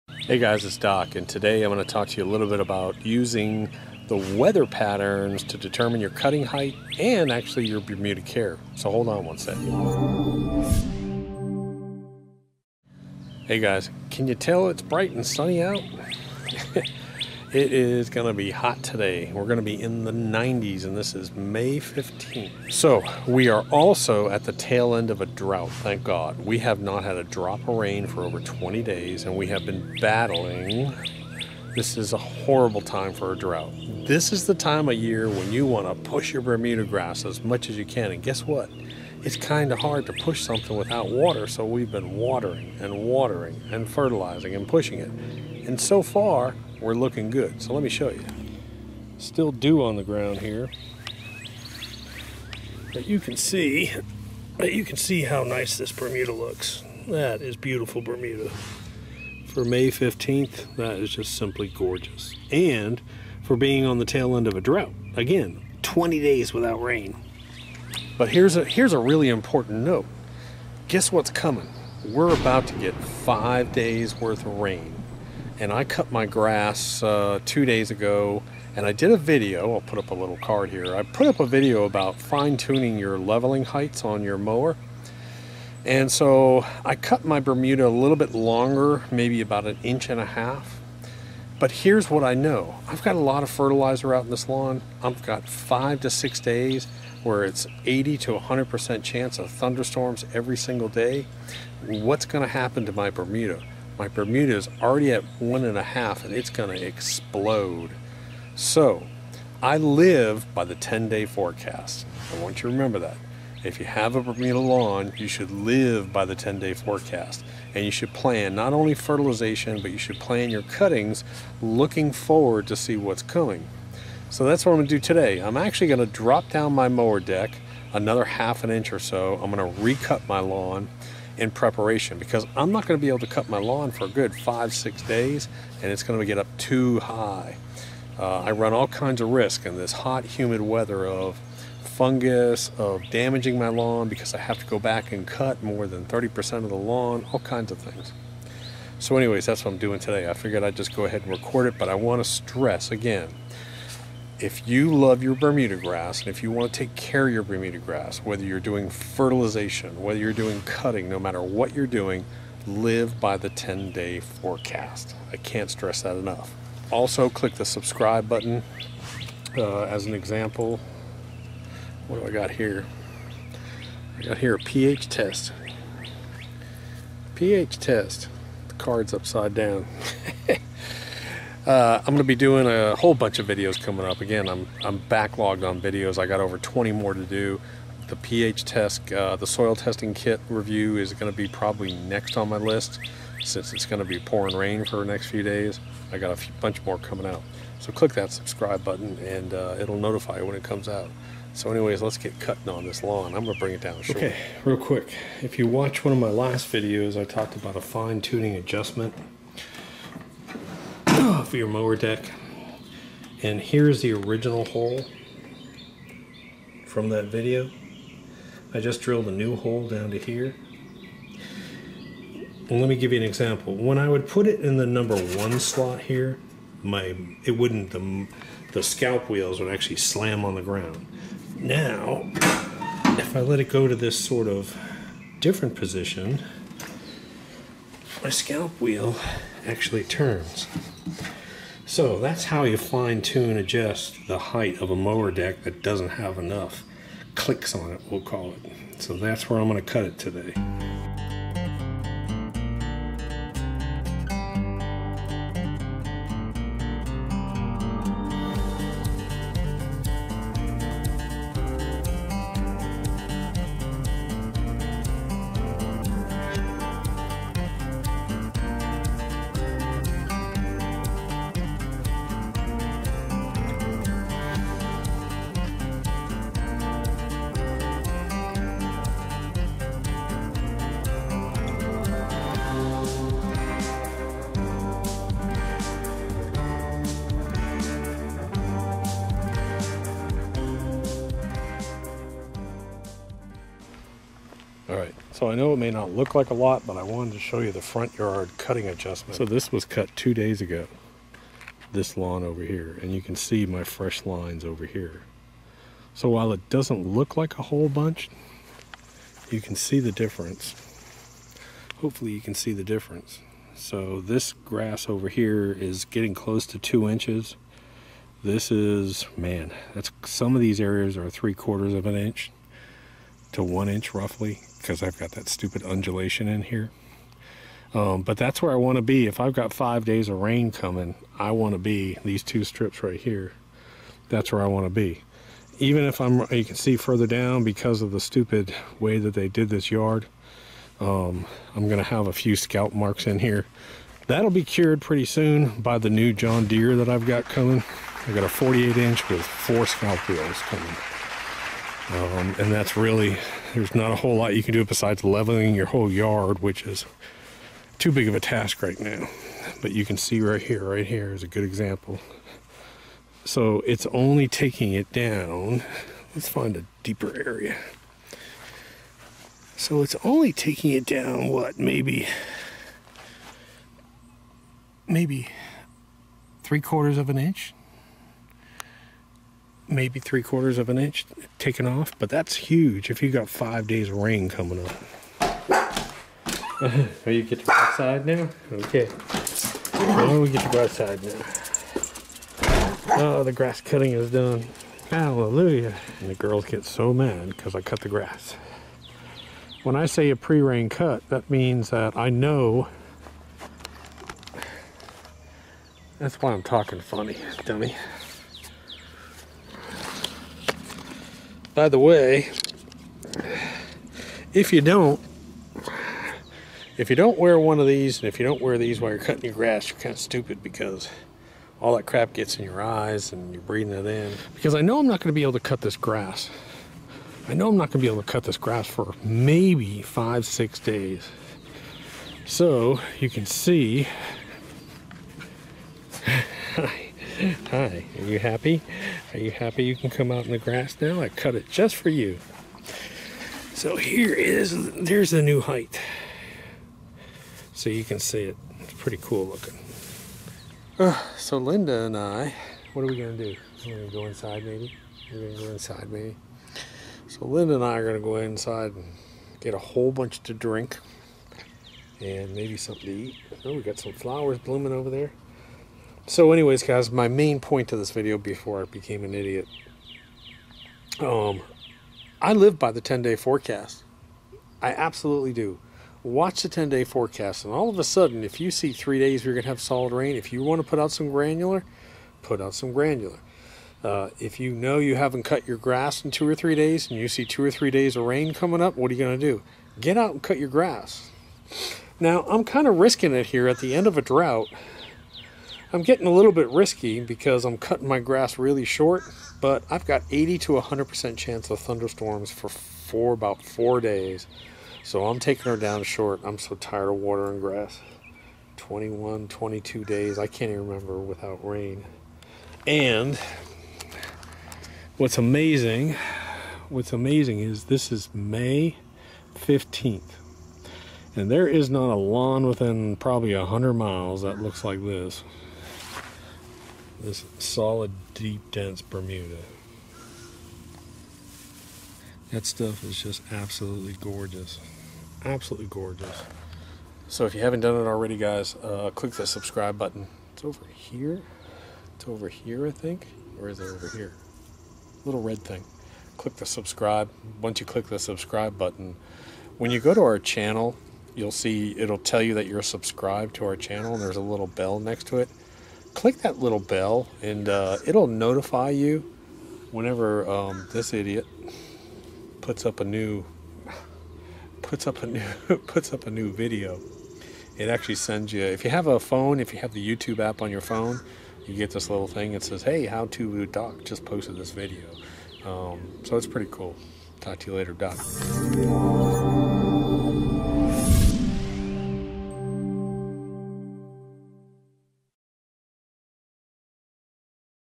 Bermuda Grass Cutting Height Change with Weather Patterns
Learn how to adjust your cutting height and general bermudagrass maintenance programs by watching the 10 day forecast.  You can't simply cut bermuda lawns the same way all the time.